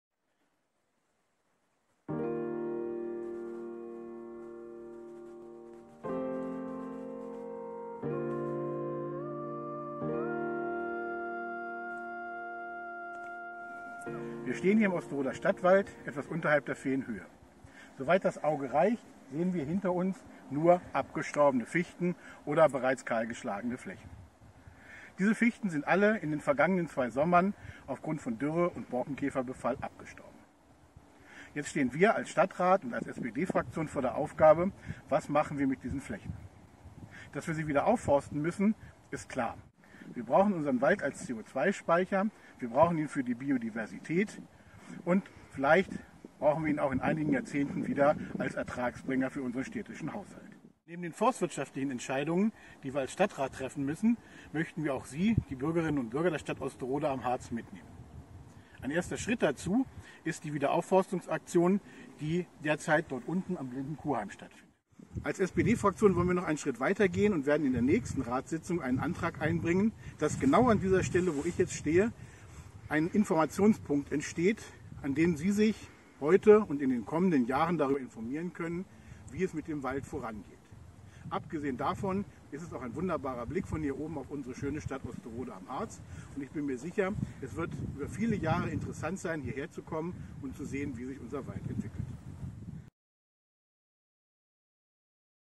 Infopunkt im Osteroder Stadtwald
Die SPD-Ratsfraktion Osterode möchte im Stadtwald einen Infopunkt errichten, an dem
SPD-Ratsfraktion Osterode / Jörg Hüddersen interessierte Bürgerinnen und Bürger über den Waldumbau informieren können.